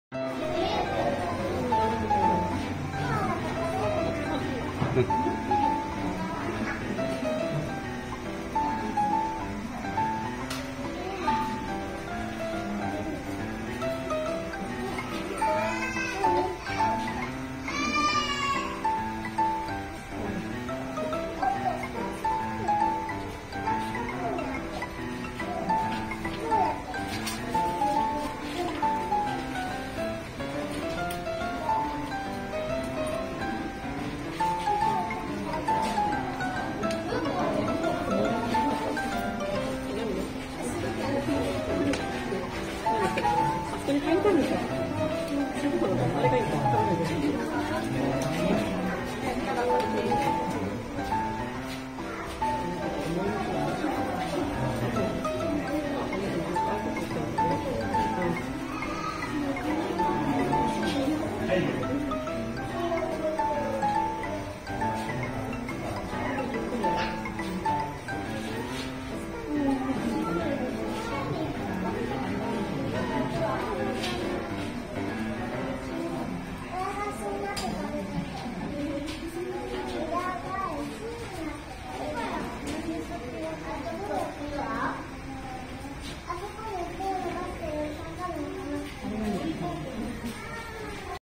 [豪華なオヤツ]コアラちゃん🐨
ユーカリが来ました、コアラちゃんあんまり食べなかっな！